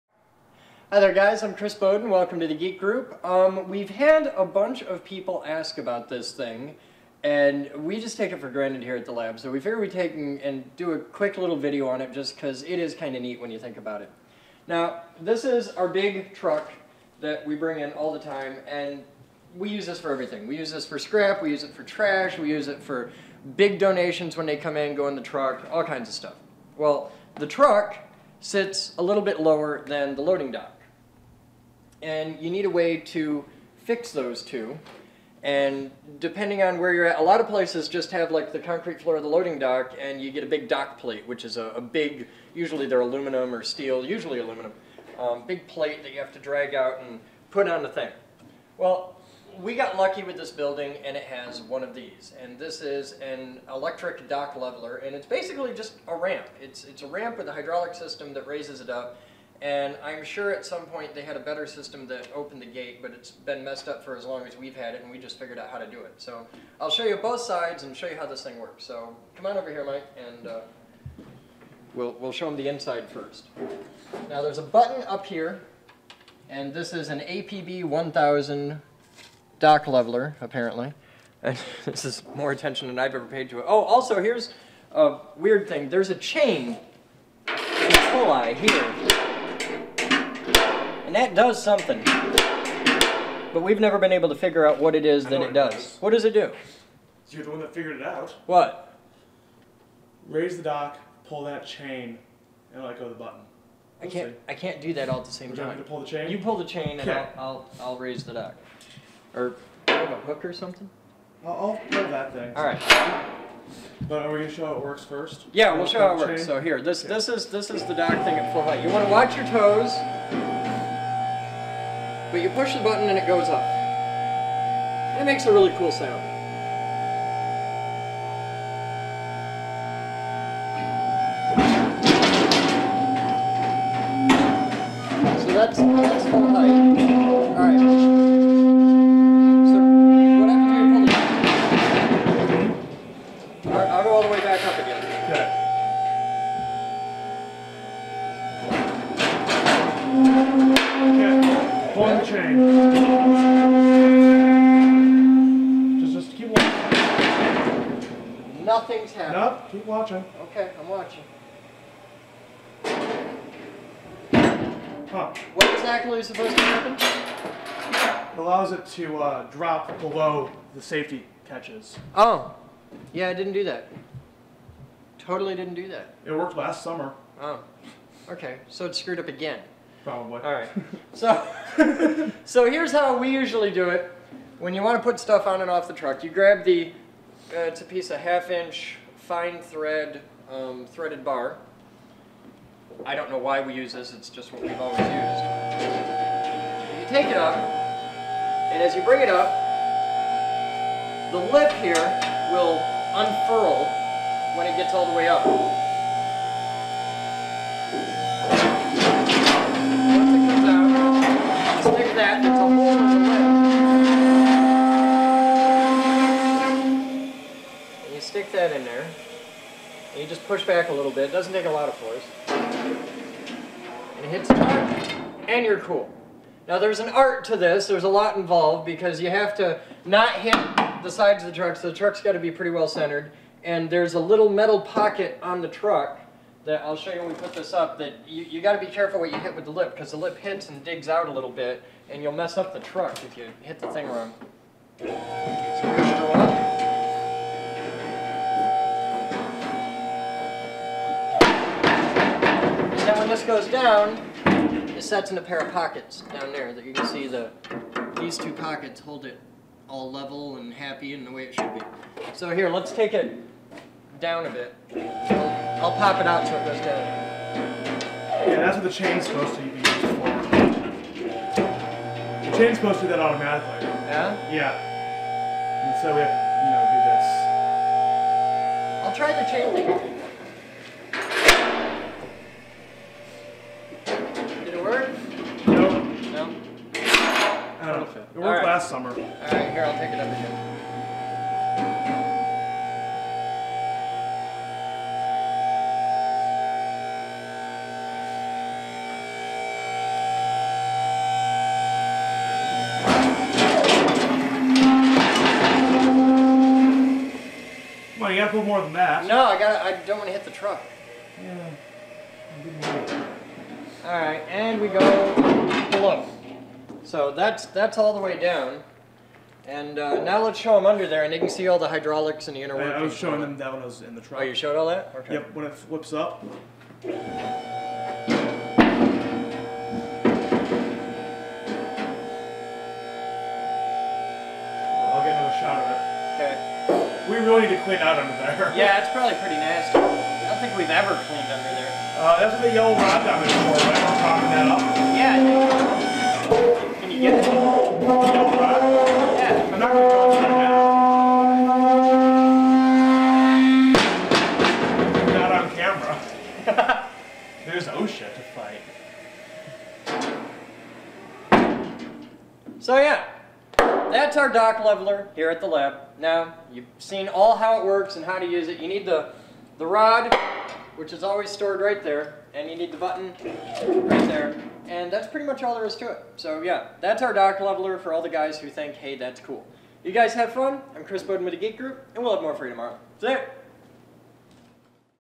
Dock Leveler
Chris and Mikey talk about the dock leveler at the Heavy Industries lab.